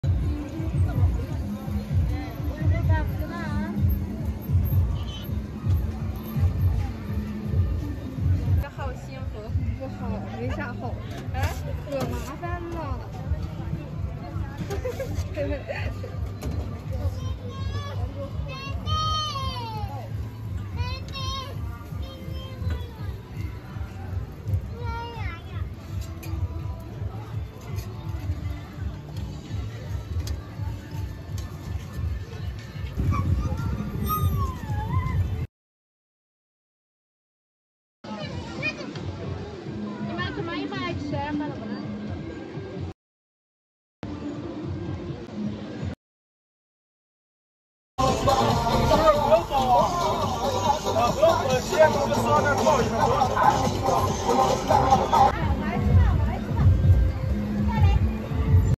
Uyghur street food in China
Uyghur street food in small Chinese town. this video I am Chinese province Inner Mongolia. I will show you the Muslim Uyghur street food. Usually Uyghurs serve lamb and beef meat